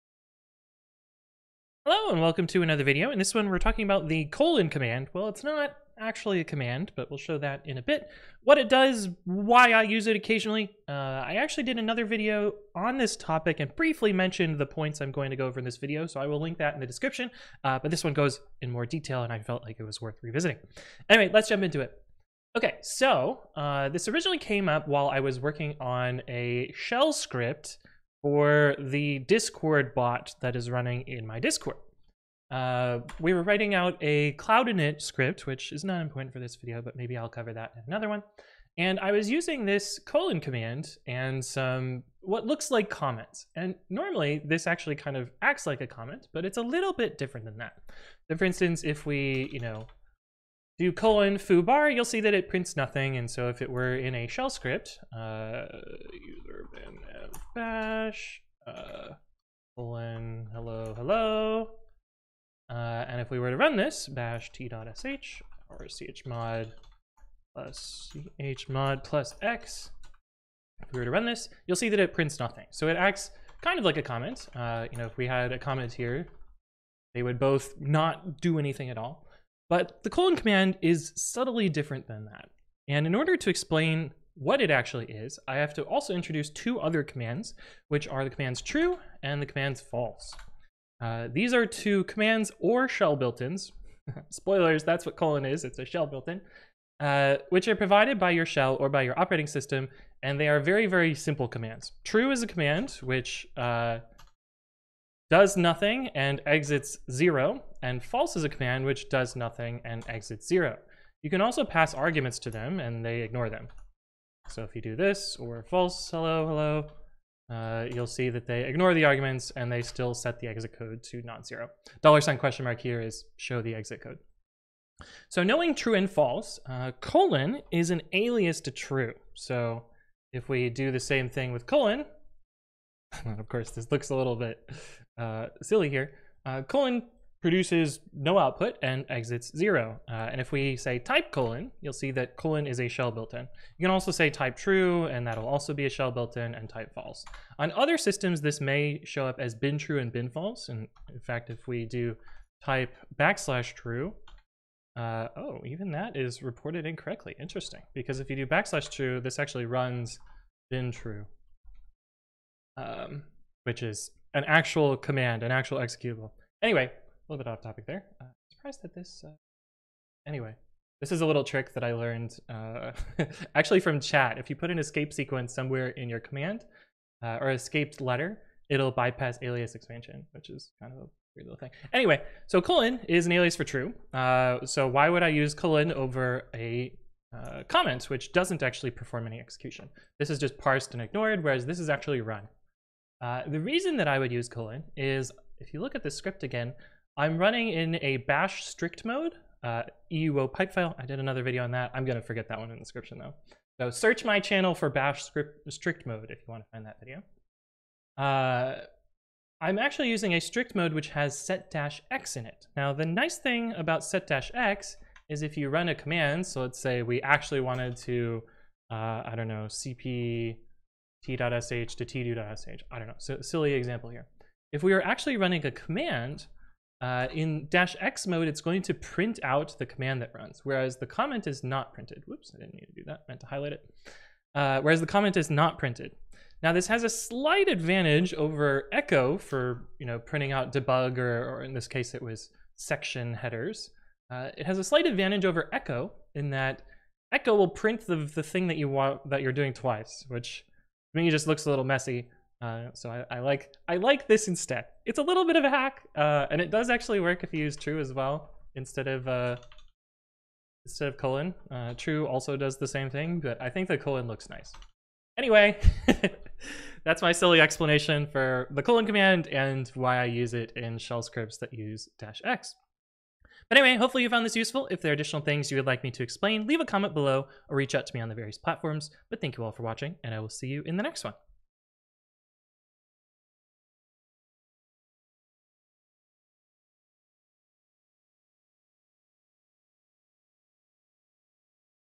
why I use the colon (:) command (intermediate) anthony explains #505
today we talk about a fairly useless command and why I like to use it in shell scripts!